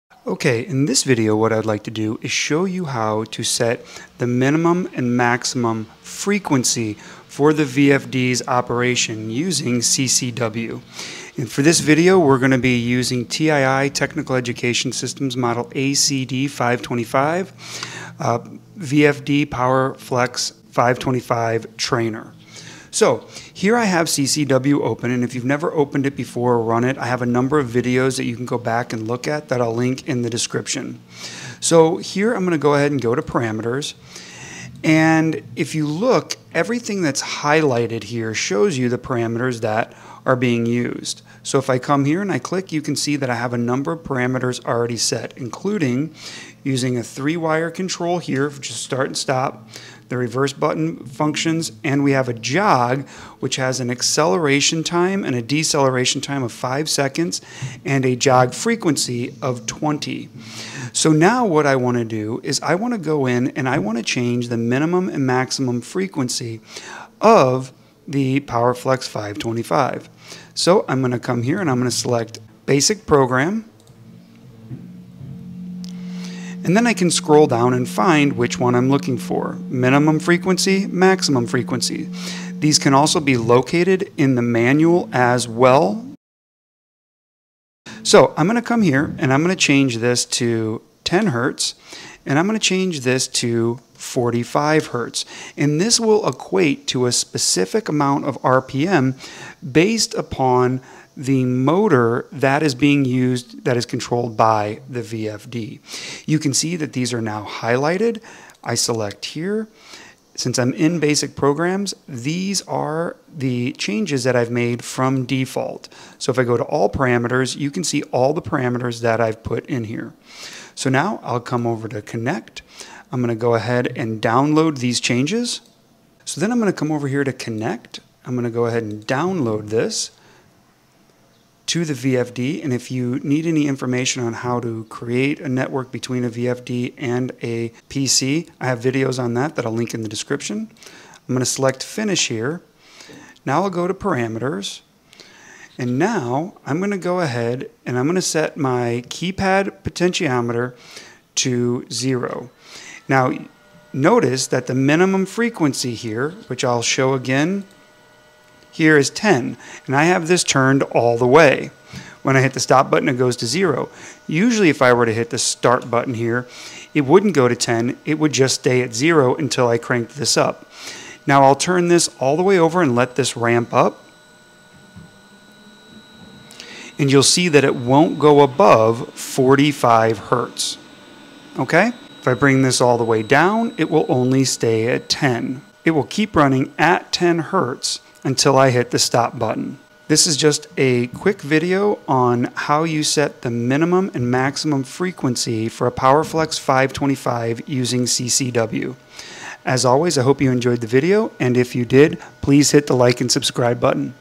Setting the Min and Max Freq on PowerFlex 525 Using CCW
In this tutorial, we guide you through the process of programming the minimum and maximum frequency settings on your PowerFlex 525 Variable Frequency Drive (VFD) using Connected Component Workbench. Correctly setting these parameters is crucial for optimizing the performance and protection of your motor and drive system.
Key topics covered:

Connecting your PowerFlex 525 VFD to Connected Component Workbench
Navigating the software interface
Setting the minimum and maximum frequency limits
Saving and applying the new configurations
This step-by-step guide is ideal for engineers, technicians, and DIY enthusiasts aiming to fine-tune their VFDs for precise control and enhanced system performance.